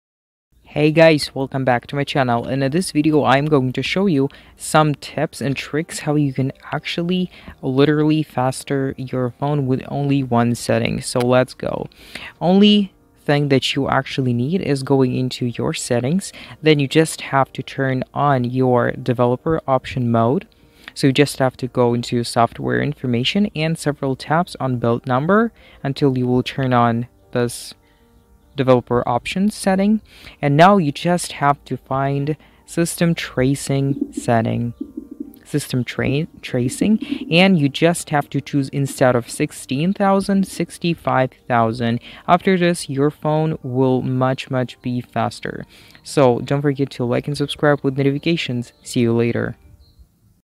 Phone Freezes? Turn On This to Stop Phone Freeze!
Hey guys! In this video I'II show you How to Stop Phone Freeze.